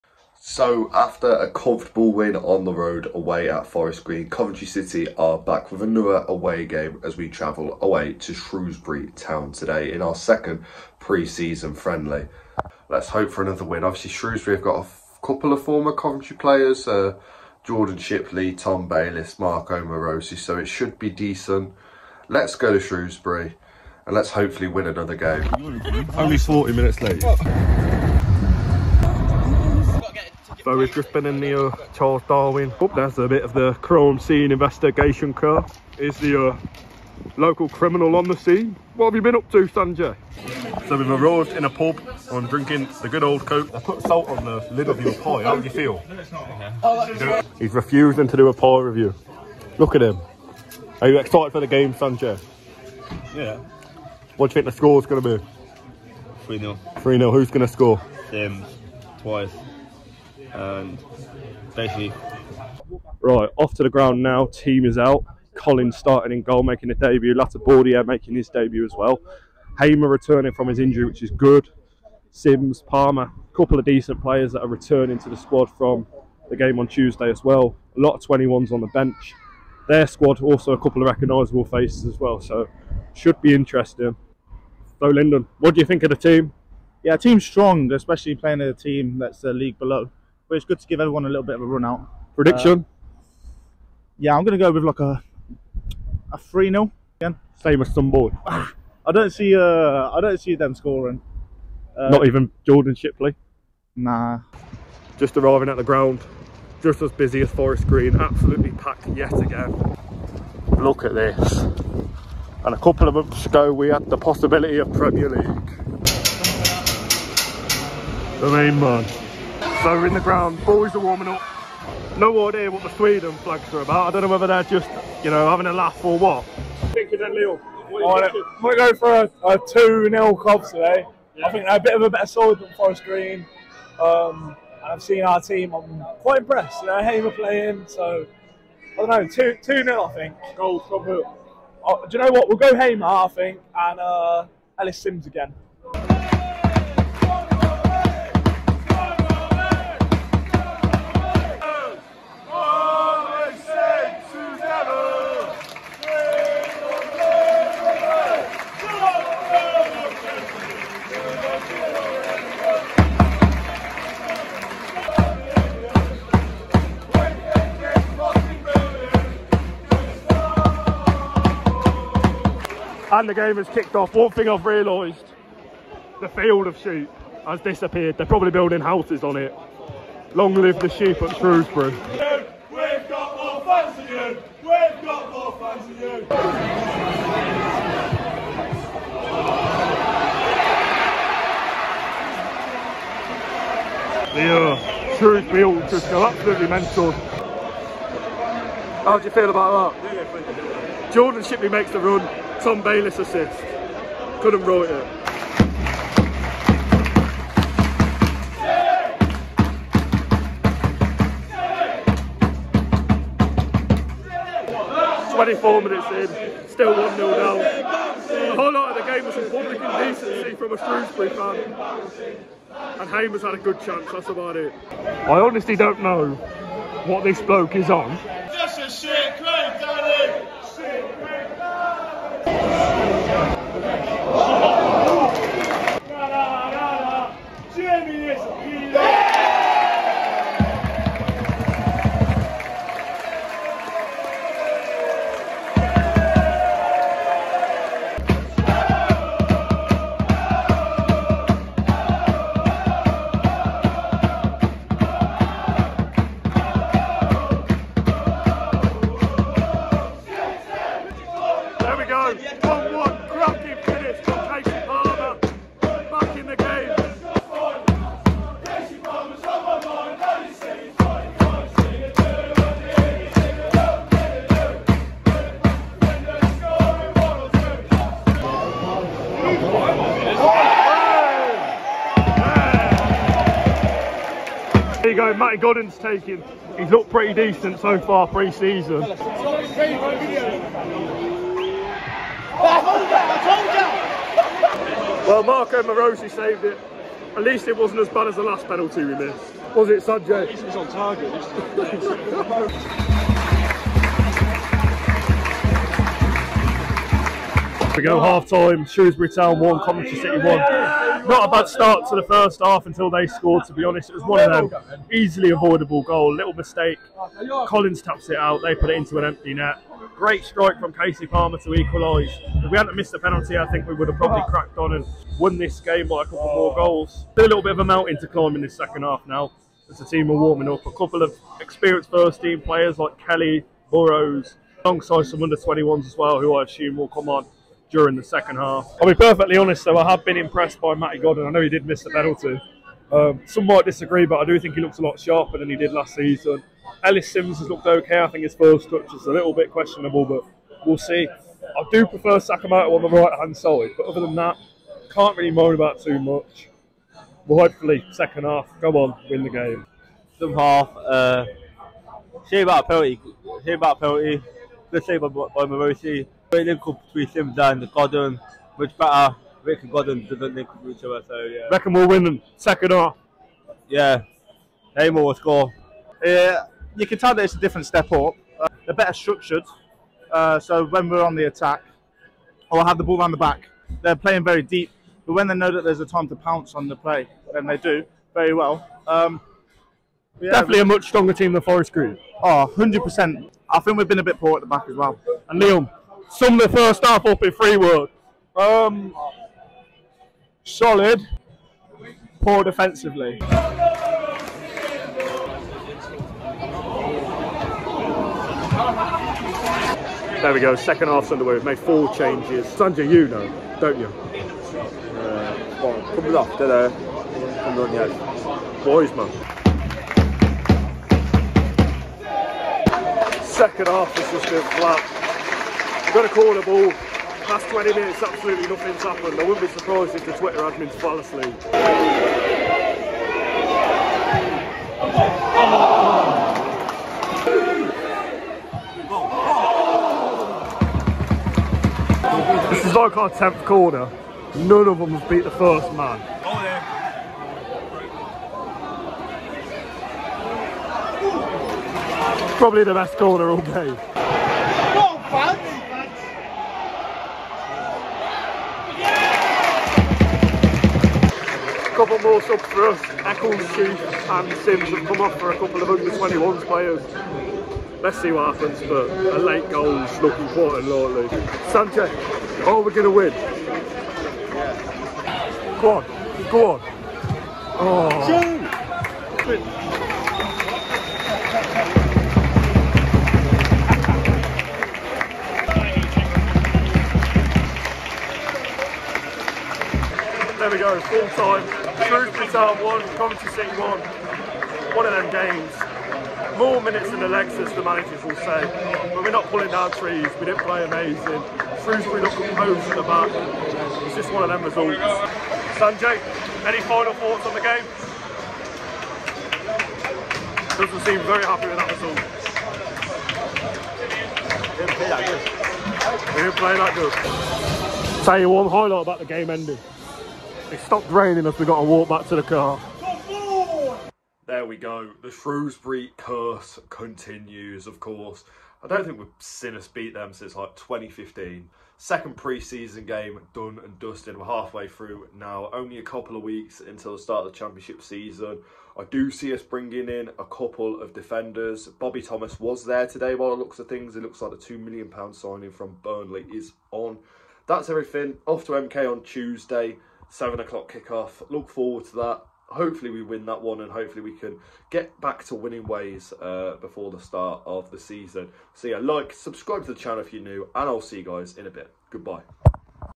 GUSTAVO HAMER RETURNS! Shrewsbury Town VS Coventry City
Coventry City head to The Croud Meadow home of Shrewsbury Town for their second pre season fixture of the 2023/24 season.

The Sky Blues were held at Croud Meadow in their second pre-season fixture against Shrewsbury Town.

City fell behind through Ryan Bowman’s strike in the first half, before Kasey Palmer equalised midway through the second half.

Mark Robins was able to hand minutes to new signings Brad Collins and Joel Latibeaudiere, as well as minutes for Gustavo Hamer.

The first chance fell to the Sky Blues when Jake Bidwell’s ball into the box was glanced on by Matty Godden, but he couldn’t steer his header on target.

However, five minutes later, with their first shot on target, Shrewsbury took the lead through Ryan Bowman, after Brad Collins had initially kept out Tom Bayliss’ effort.

City looked for an immediate response and they almost had it if it wasn’t for a fine save from Marko Marosi to keep out Ben Sheaf’s strike from the edge of the box.